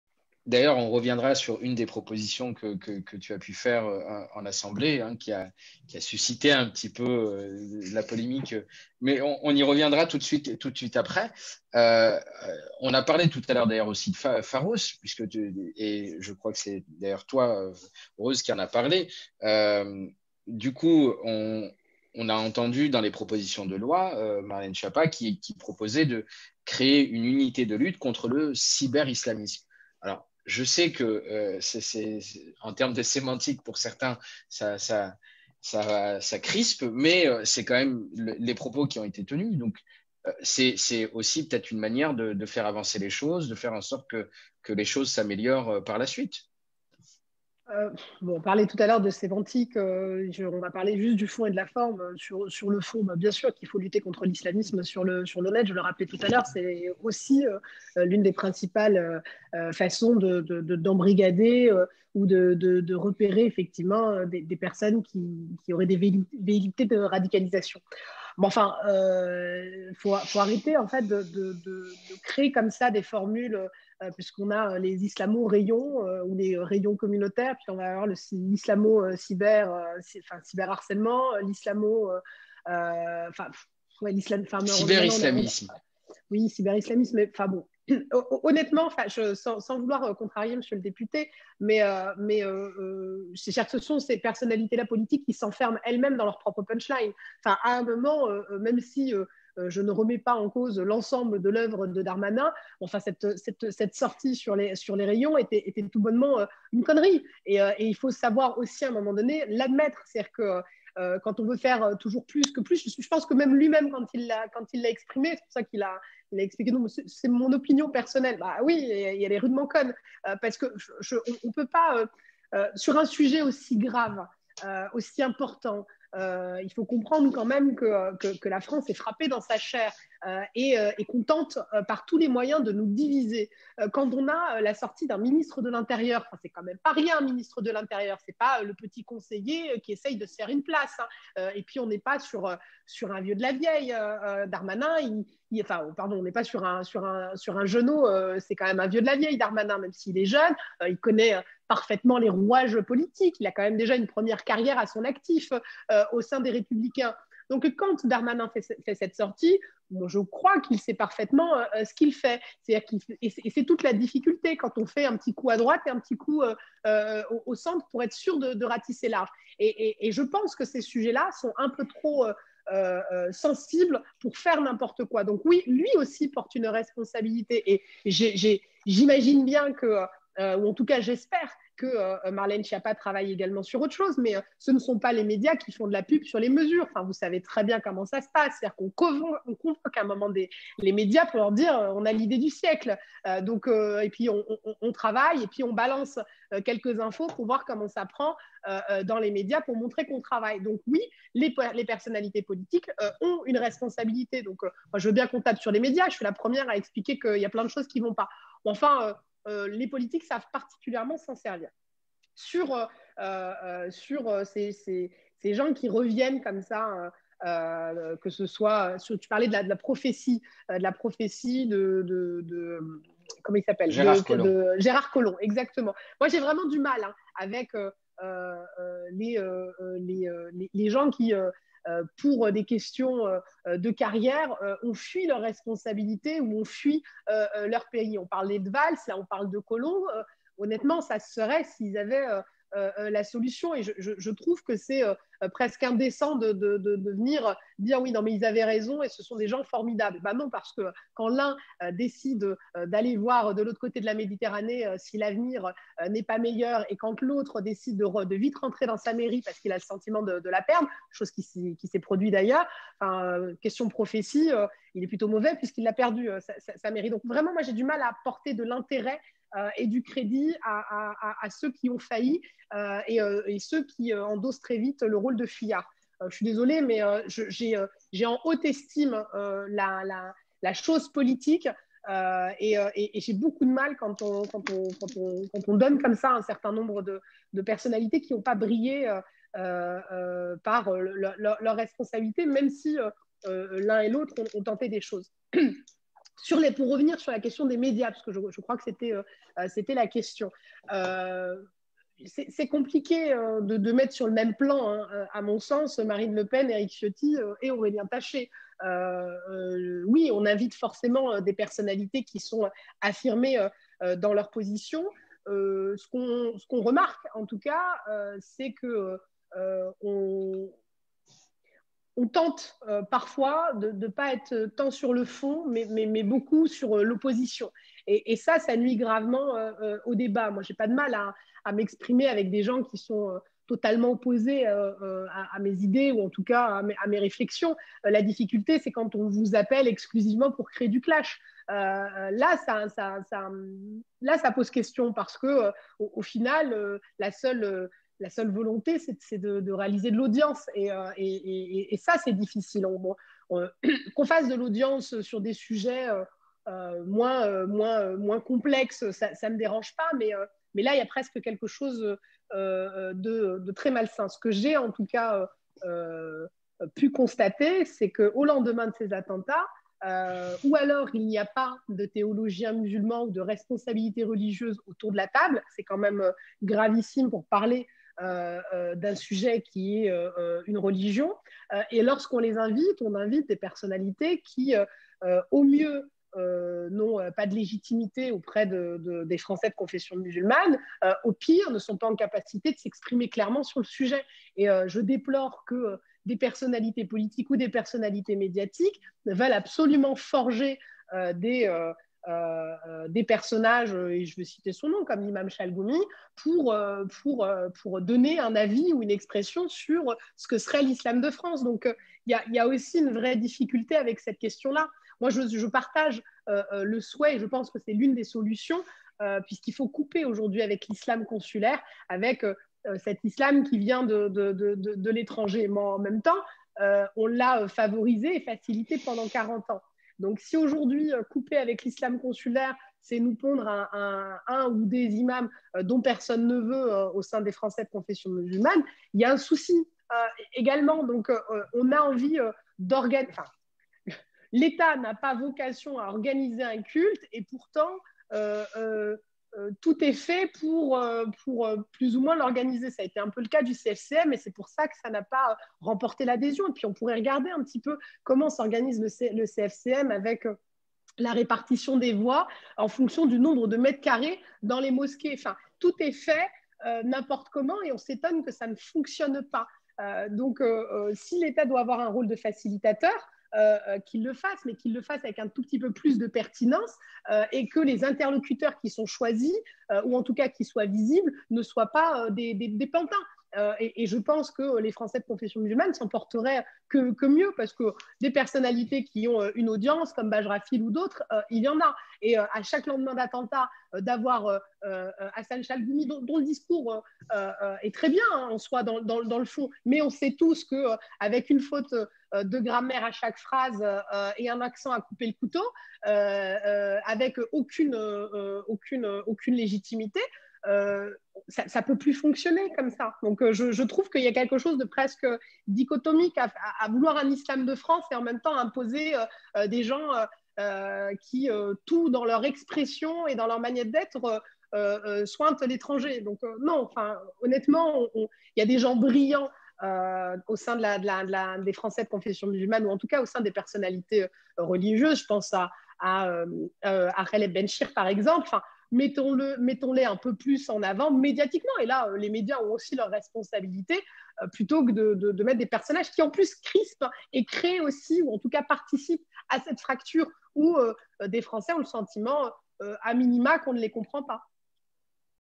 L'islam, la politique et les médias - Rose Ameziane
Rose Ameziane sur Direct citoyens le 01 novembre 2020 présentée par Jaafar Greinch en compagnie de Rose Ameziane (présidente du Mouvement pour l'émancipation des territoires et chroniqueuse politique), Ludovic Mendes (député LREM de Moselle) et Fanny Bauer-Motti (psychologue et podcasteuse).